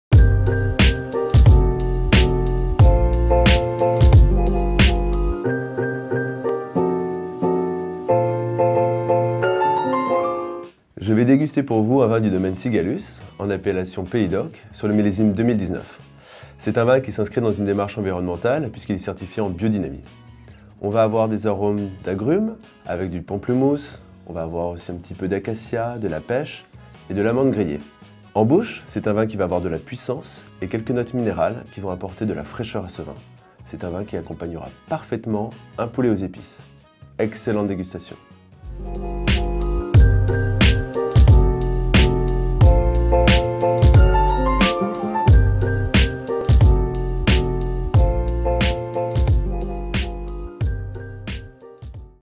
Domaine de Cigalus Gérard Bertrand, Cigalus Blanc 2019
Frédéric, notre sommelier vous fait parvenir la cuvée Cigalus Blanc 2019 du Domaine de Cigalus -Gérard Bertrand.